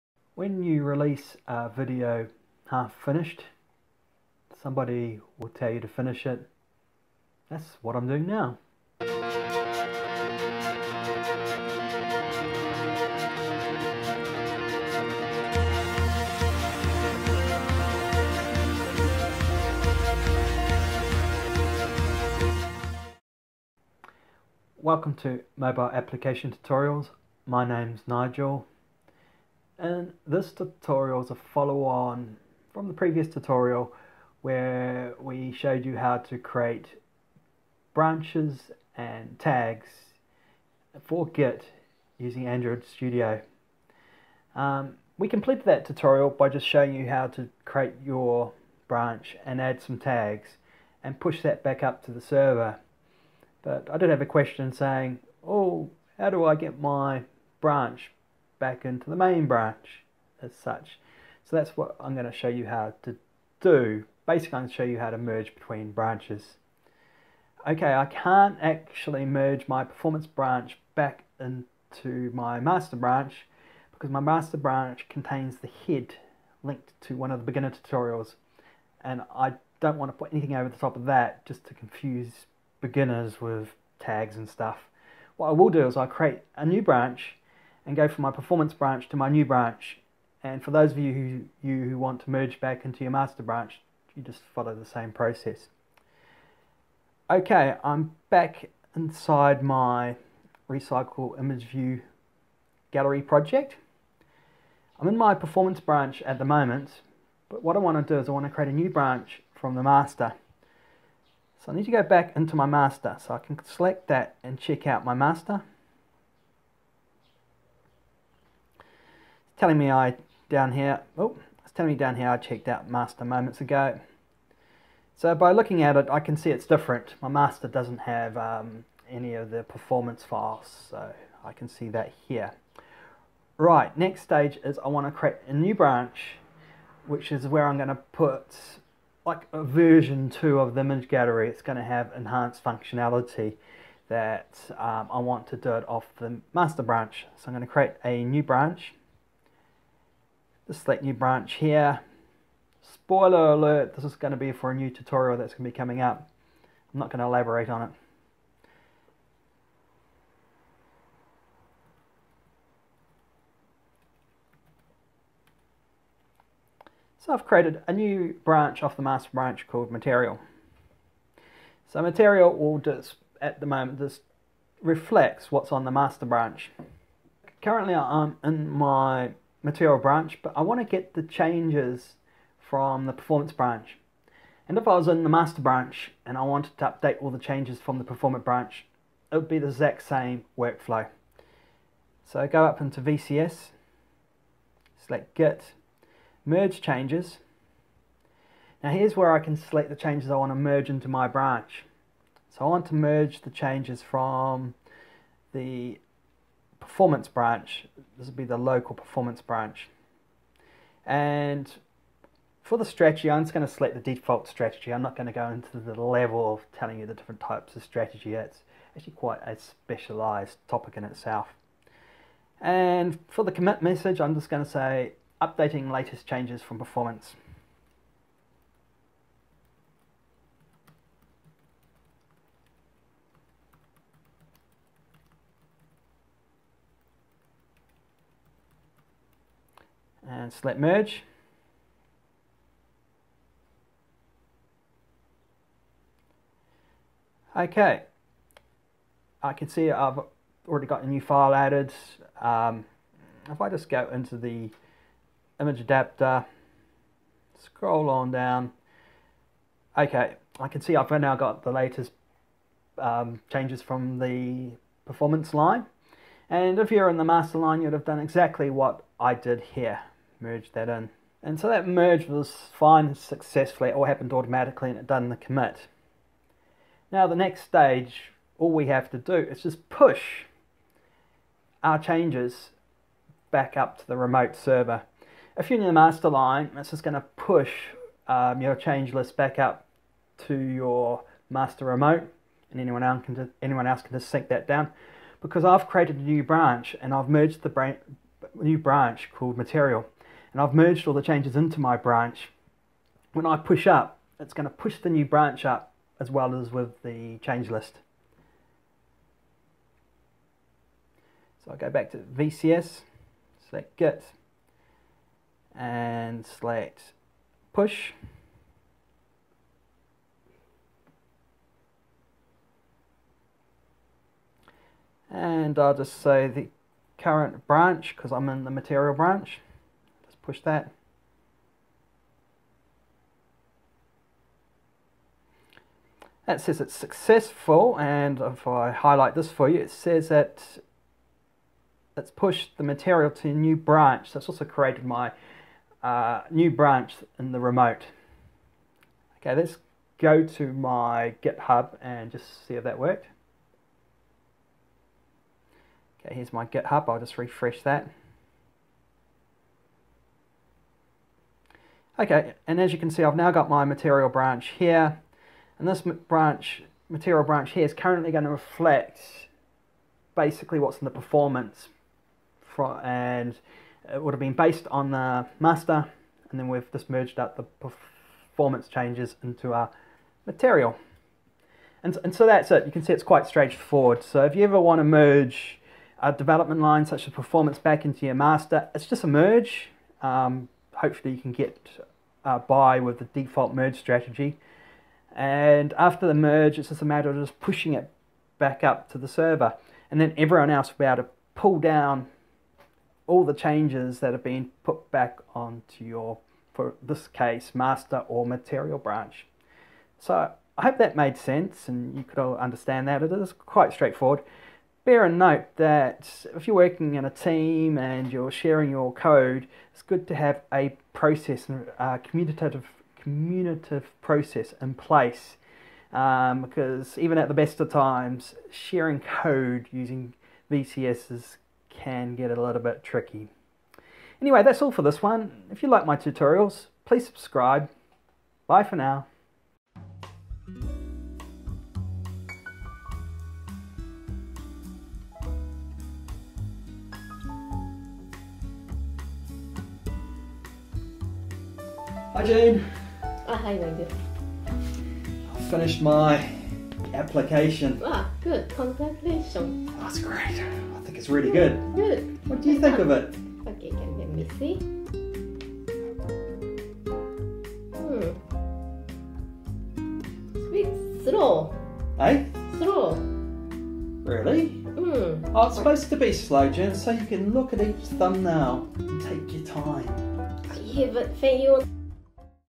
How to merge between git branches using android studio
where I will be showing you have to merge your code changes between branches and then push the changes back up to the GitHub repo.

I start off by creating a new branch based off the main master branch. I then merge the changes from the performance branch to my new branch and then push those changes(commits) up to the GitHub repo which will also create a new remote branch.

This workflow could be used as an example of how to merge changes back into the main code-line prior to pushing them up to the repo therefore making the code available to team members.